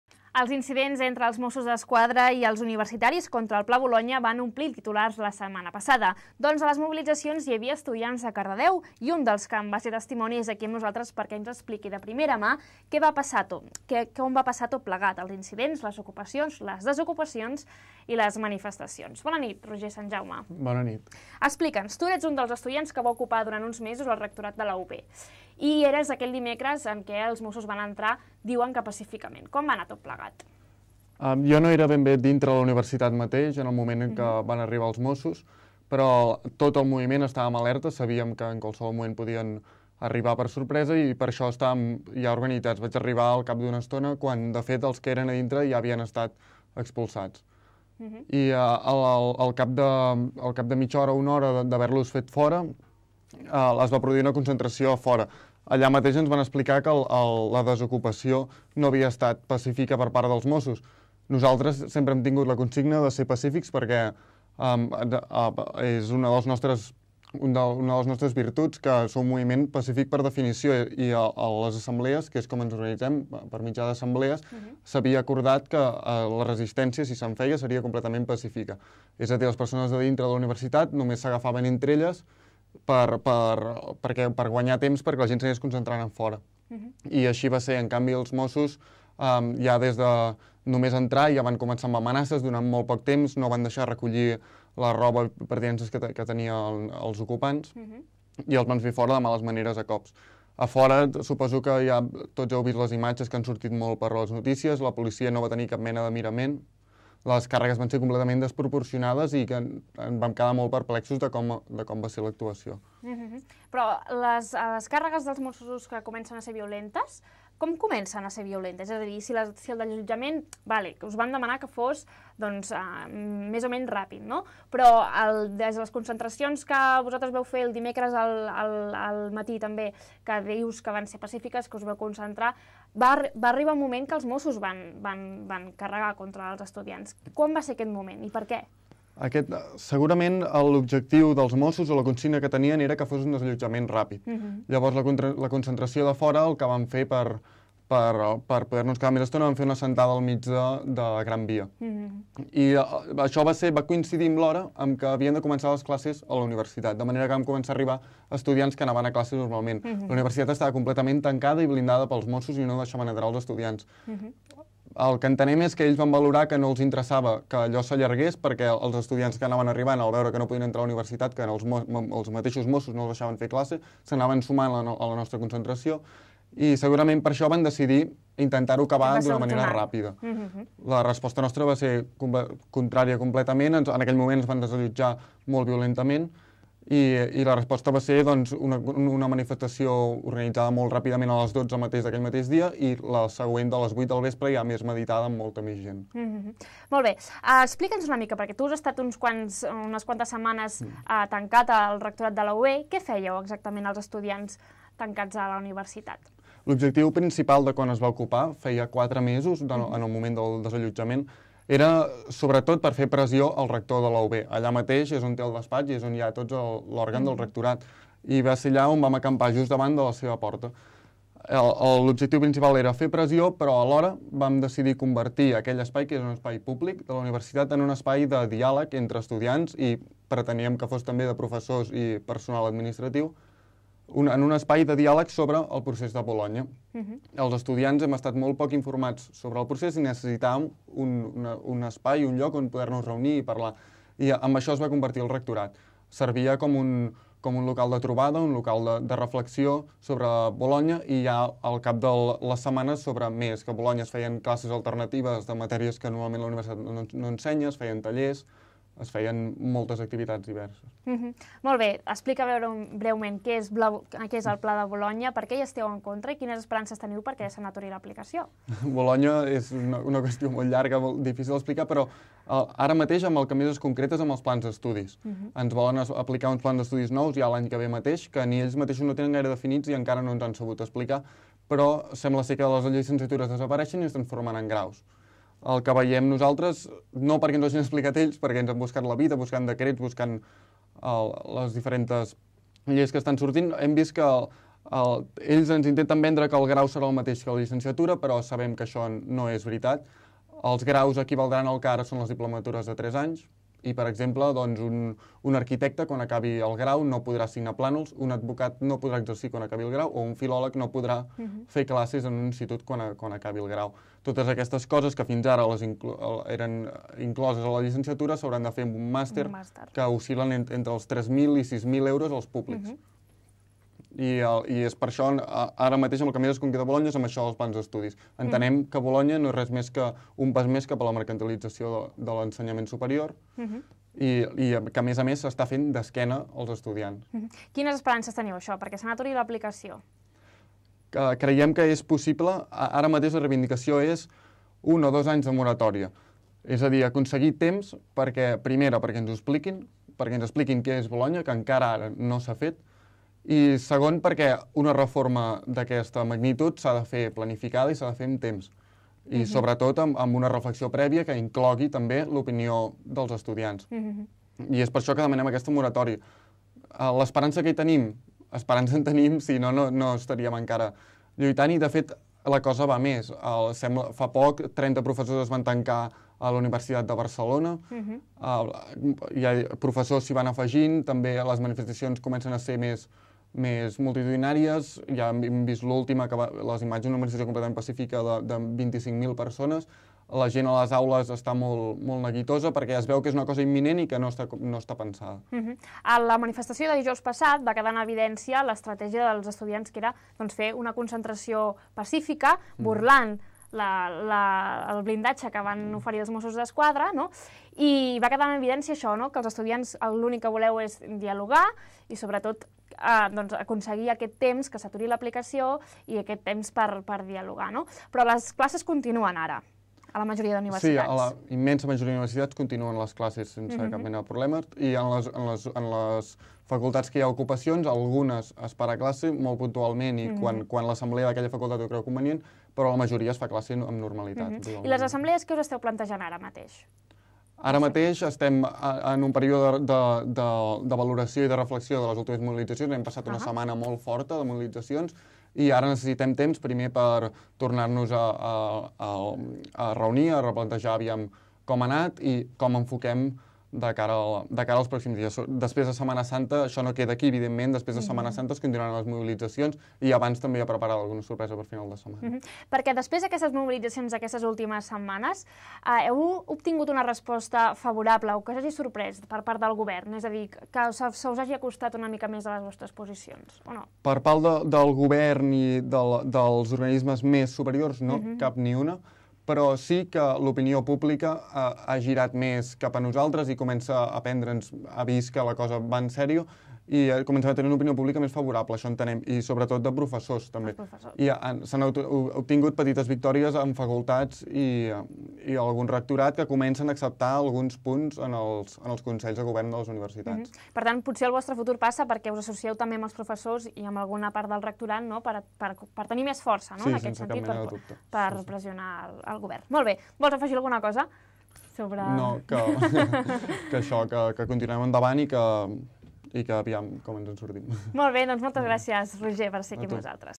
Cardedeuencs a la càrrega dels mossos a la UB
Els incidents entre els mossos d'esquadra i els universitaris contra el Pla Bolonya van omplir titulars la setmana passada.
Doncs a les mobilitzacions hi havia estudiants de Cardedeu i un dels que en va ser testimoni, és aquí amb nosaltres perquè ens expliqui de primera mà com va passar tot plegat, dels incidents, de les okupacions i desokupacions i de les manifestacions.